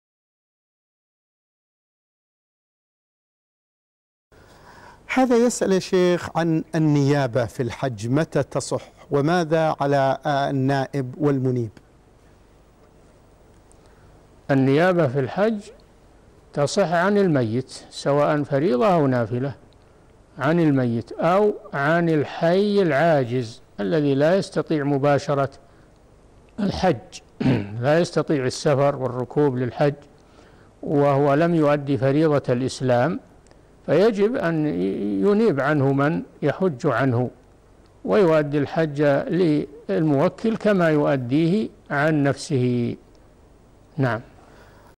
متى تصح النيابة في الحج؟ الشيخ صالح الفوزان
"نص السؤال: هَٰذَا يسال يا شيخ: عن النيابة في الحج متى تصح، وماذا على النائب والمنيب؟
السيرة الذاتية :
للاشتراك في قناة الشيخ صالح الفوزان
لمشاهدة حلقات فتاوى على الهواء كاملة

للاستفسار والإقتراحات والدعم
رئيس الفريق
لمشروع كبار العلماء